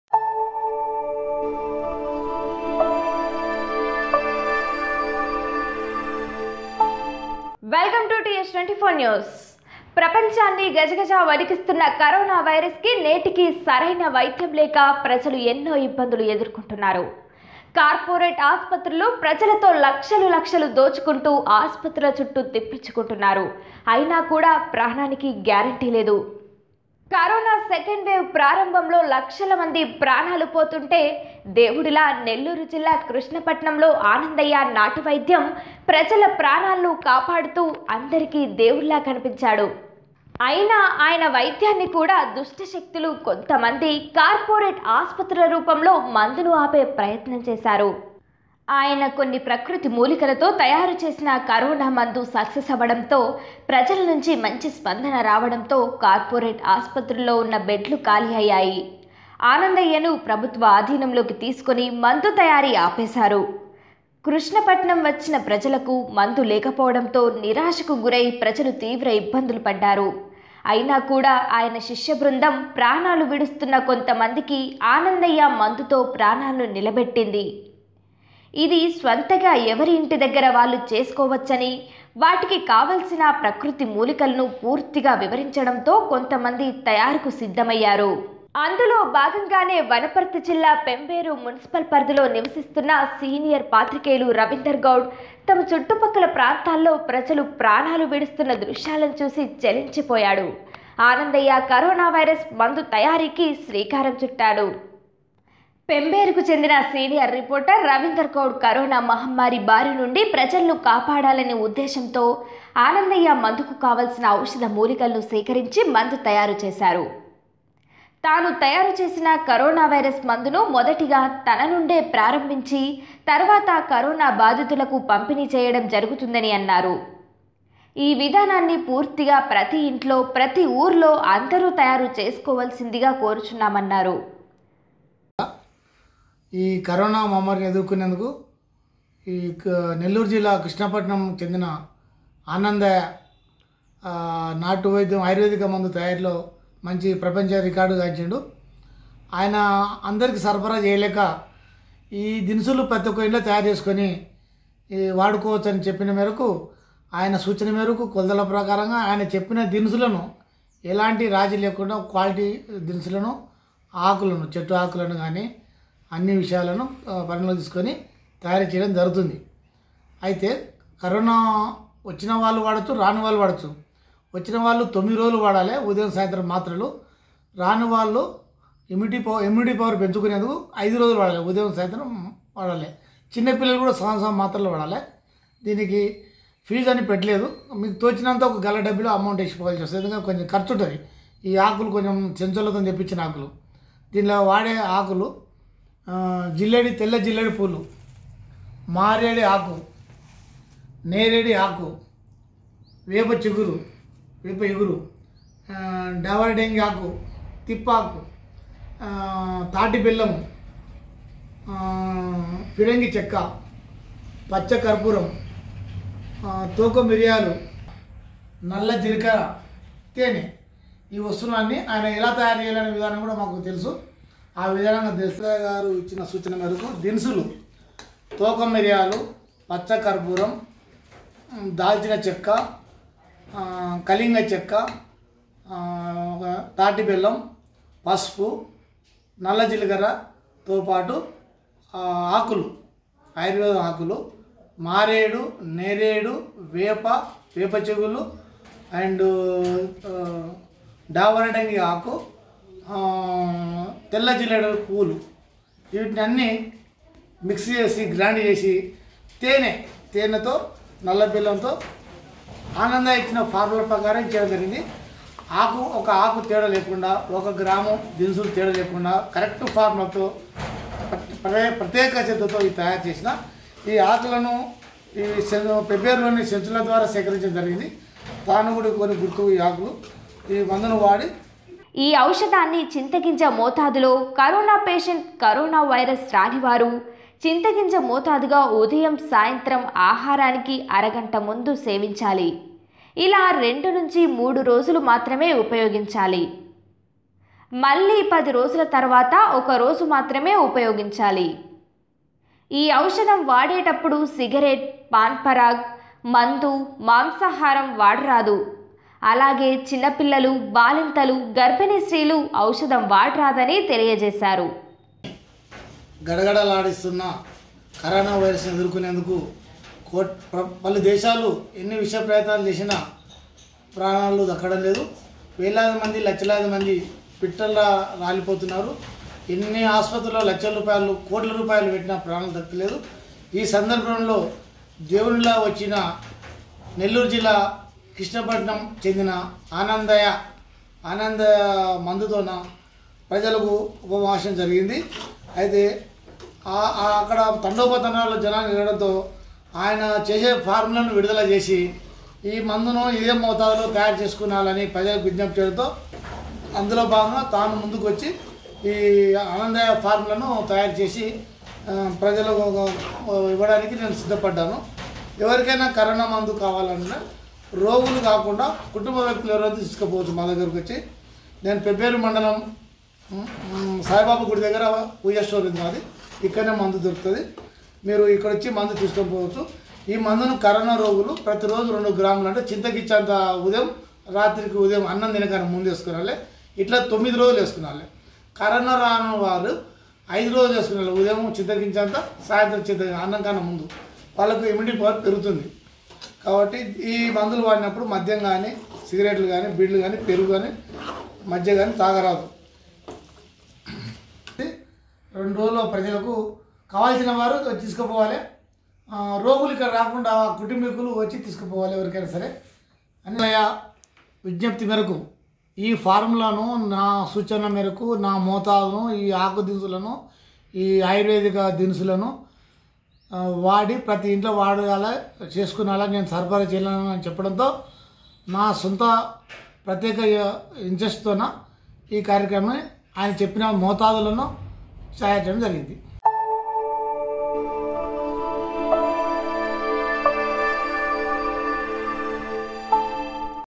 లక్షలు తీసుకుంటున్న కార్పొరేట్ ఆస్పత్రికి దీటుగా కరోనా మందు|TS24 News
ప్రపంచాన్ని గజగజ వణికిస్తున్న కరోనా వైరస్ నేటికీ సరైన వైద్యం లేక ప్రజలు ఎన్నో ఇబ్బందులు ఎదుర్కొంటున్నారు.
కార్పొరేట్ ఆసుపత్రులు ప్రజలతో లక్షలులక్షలు దొబ్బుతూ ఆస్పత్రుల చుట్టూ తిప్పించుకుంటున్నారు. అయినా కూడా ప్రాణానికి గ్యారెంటీ లేదు.
కరోనా సెకండ్ వెవ్ ప్రారంభంలో లక్షల మంది ప్రజలు ప్రాణాలు పోతుంటే. దేవుడిలా నెల్లూరు జిల్లా కృష్ణపట్నం లో ఆనందయ్య నాటు వైద్యం ప్రజల ప్రాణాలను కాపాడుతూ అందరికీ దేవుడిలా కనిపించాడు. అయినా ఆ దేవుడిని కూడా దుష్ట శక్తుల కొంతమంది కార్పొరేట్ ఆసుపత్రుల రూపంలో ఆయన మందును ఆపే ప్రయత్నం చేశారు.
ఆయన కొన్ని ప్రకృతి  మూలికలతో తయారు చేసిన కరోనా మందు సక్సెస్ అవడంతో  ప్రజల నుంచి మంచి స్పందన రావడంతో కార్పోరేట్ హాస్పటల్ లో ఉన్న బెడ్లు ఖాళీ అయ్యాయి... ఆనందయ్య ను ప్రభుత్వ  ఆధీనంలోకి తీసుకుని మందు తయారీ ఆపేశారు. కృష్ణ పట్నం వచ్చిన ప్రజలకు మందు లేకపోవడంతో, నిరాశకుగురై ప్రజలు తీవ్ర ఇబ్బందులు పడ్డారు. అయినా కూడా  ఆయన శిశ్యు బృందం ప్రాణాలు విడుస్తున్న కొంత మందికి  ఆనందయ్య మందుతో ప్రాణాలను నిలబెట్టింది. ఇది స్వంతంగా ఎవరి ఇంటి దగ్గర వాళ్లు చేసుకోవచ్చునని, వాటికి కావాల్సిన ప్రకృతి మూలికలను పూర్తిగా వివరించడంతో కొంతమంది తయారు కు సిద్ధమయ్యారు.
అందులో భాగంగానే  వనపర్తి జిల్లా పెబ్బేరు మున్సిపల్ పరిధిలో నివసిస్తున్న సీనియర్ పాత్రికేయులు రవీందర్ గౌడ్ తమ చూట్టూ పక్క ప్రాంతాల్లో ప్రజలు ప్రాణాలు విడుస్తున్న దృశ్యాలను చూసి చలించిపోయి...
ఆనందయ్య కరోనా వైరస్ మందు తయారీ కి శ్రీకారం చుట్టారు.
పెబ్బేరు కు చెందిన సీనియర్ రిపోర్టర్ రవీందర్ గౌడ్  కరోనా మహమ్మారి బారి నుండి ప్రజలను కాపాడాలని ఉద్దేశ్యంతో ఆనందయ్య మందు కు కావలసిన ఔషద మూలికలను  సేకరించి  మందు తయారు చేశారు.

తాను తయారు చేసిన కరోనా వైరస్ మందును మొదటగా తన నుండే ప్రారంబించి తర్వాత కరోనా బాధితులకు పంపిణి చేయడం జరుగుతుందని  అన్నారు.

 ఈ విధానాన్ని
పూర్తిగా, ప్రతి ఇంట్లో ప్రతి ఊరిలో అందరూ తయారు చేసుకోవాల్సిందిగా కోరుచున్నాము.
ఆయుష్ డిపార్ట్మెంట్ వారు కూడా ఔషధం లో ఎలాంటి దుష్ప్రభావాలు లేనందున  ఈ మందు అందరూ వాడవచ్చు.

 ఈ ఔషధం తయారీ విధానాన్ని ఆయుష్ డిపార్ట్మెంట్ వారు ఇచ్చిన మోతాదులో మనం తయారుచేయడం జరిగినది.
 కాబట్టి ఈ ఔషధం ప్రతి ఇంట్లోనూ ప్రతి ఊళ్ళలో
అన్నిచోట్ల దొరికే వనమూలికలు, మూలికా దినుసులు ప్రతి ఆయుర్వేదం షాప్ లోను లభిస్తాయి.
ఆనందయ్య గారు ఆయుష్ డిపార్ట్మెంట్ వారికి ఇచ్చిన ఔషధాలు మోతాదు లో మనం ఔషధాన్ని తయారు చేయడం జరిగినది . ఈ ఔషధం లో వాడిన వన మూలికలు.. పేర్లు వాటి మోతాదు.
తెల్ల జిల్లేడు పూలు 200గ్రా
 మారేడు  ఇగురు 200 గ్రా
 నేరేడు ఇగురు 200గ్రా
 వేప ఇగురు 200గ్రా
డావరడంగి ఆకులు 200గ్రా
 నల్ల జీలకర్ర 30గ్రా.
పట్టా దాల్చిన చెక్క30.
  పసుపు 30.గ్రా
తోక మిరియాలు గ్రా30.
పచ్చ కర్పూరంగ్రా30 .
ఫిరంగి చెక్క30గ్రా .
తేనె200 గ్రా.
తాటి బెల్లం200గ్రా.
పైన ఇచ్చిన మోతాదులో పూర్తిగా మిక్సీ కి వేసి పేస్ట్ చేసి ఒక నాలుగు కేజీల గిన్నెలో పదార్థాలు మొత్తం వేసి స్టౌ పైన  పెట్టి ఈ పదార్థాలు మొత్తం లేహ్యం ముద్ద లాగా అయ్యేవరకు ఉంచి వీలైతే ఒకరోజు ఎండలో పెట్టవచ్చు బూజు పట్టకుండా పాడవకుండా ఉంటుంది.  ఈ ఔషధాన్ని చింతగింజ మోతాదులో కరోనా పేషెంటు కరోనా వైరస్ రానివారు చింతగింజ మోతాదుగా ఉదయం సాయంత్రం ఆహారానికి అరగంట ముందు సేవించాలి ఇలా 2 నుంచి మూడు రోజులు మాత్రమే ఉపయోగించాలి.
 మళ్లీ పది రోజుల తర్వాత ఒకరోజు మాత్రమే ఉపయోగించాలి
 ఈ ఔషధం వాడేటప్పుడు సిగరెట్ పాన్ పరాగ్ మందు మాంసాహారం వాడరాదు అలాగే చిన్నపిల్లలు బాలింతలు గర్భిణి స్త్రీలు ఔషధం వాడరాదని తెలియాజేశారు.